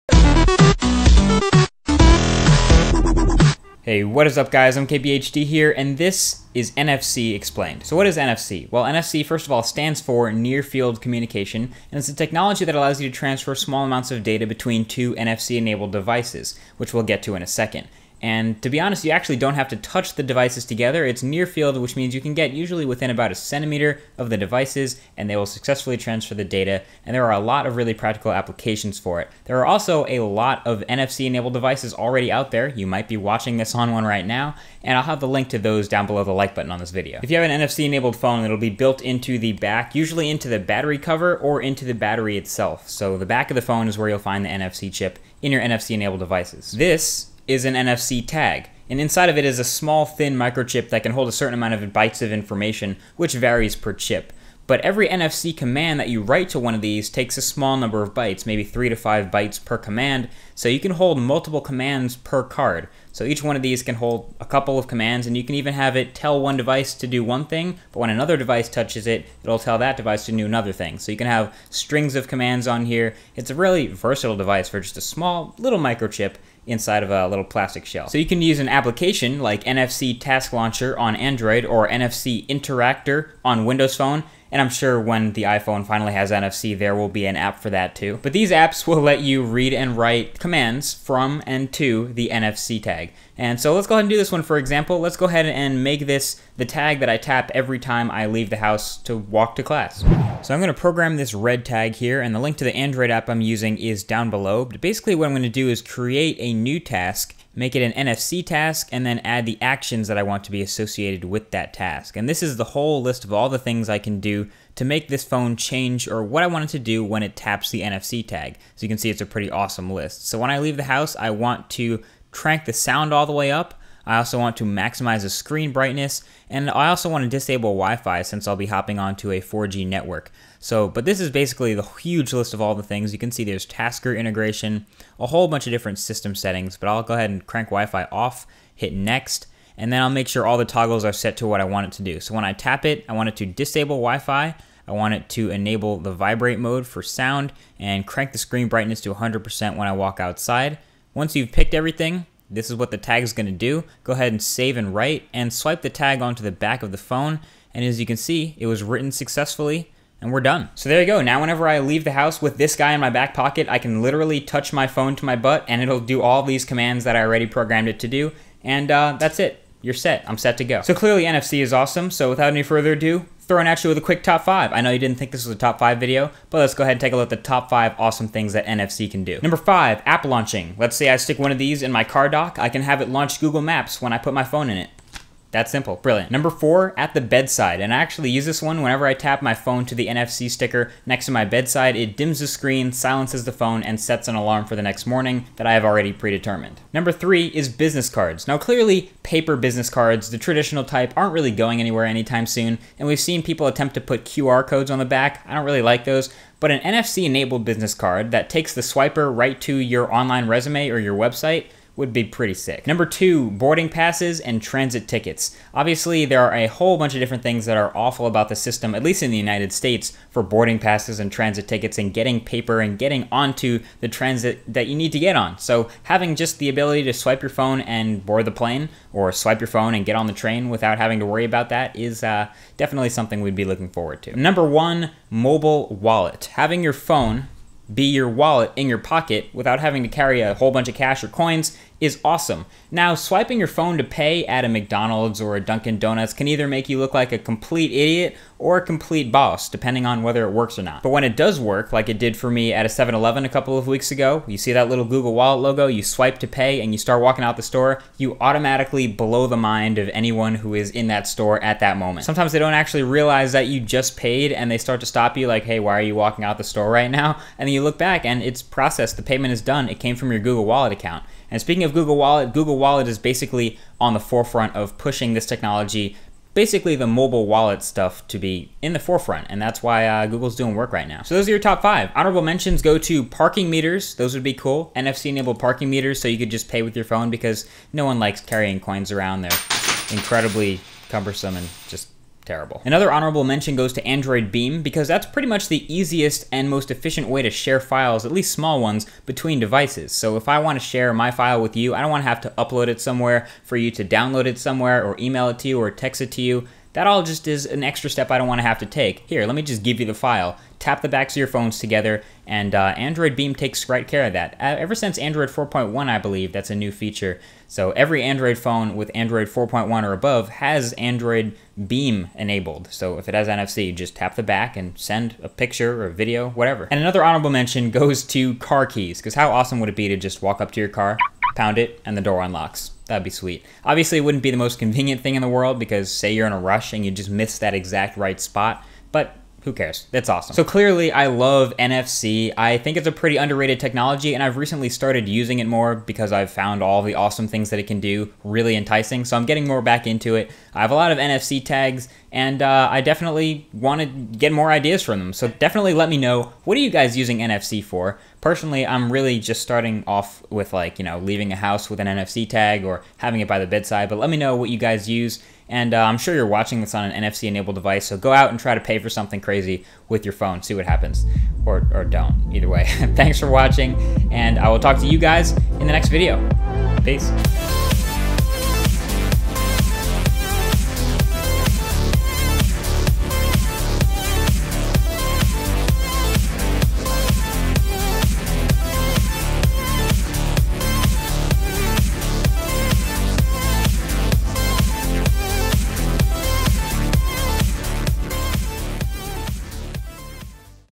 Top 5 NFC Features: Explained!
NFC Technology Explained - Plus a quick Top 5! Thumbs up!

Top 5 Awesome NFC Stuff
5. App launching [2:59]
4. Settings toggles [3:05]
3. Business Cards [3:15]
2. Transit Tickets [3:32]
1. Mobile Wallet [3:53]

Honorable mentions: NFC Parking Meters, Android Beam, & NFC Car keys!